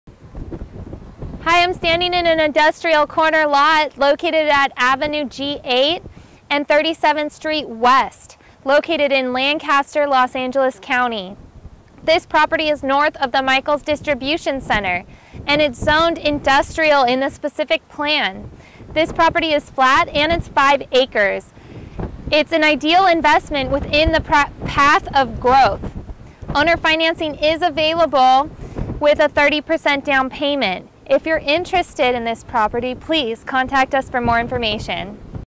Industrial Land For Sale, Lancaster, CA
Land for sale,  Ave G8+ 37th ST west., Lancaster, Los Angeles County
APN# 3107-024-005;

This is an Industrial corner lot 5 Acre in size with a road frontage.
Its fronting the Ave G8+ 37th St West in Lancaster Los Angeles County
North of Michael’s Distribution center.
The zoning is Industrial in the specific plan, its flat, size is over 5 Acres, This is an ideal investment lot within the path of growth.

Owner finance is available on this lot with 30% down payment.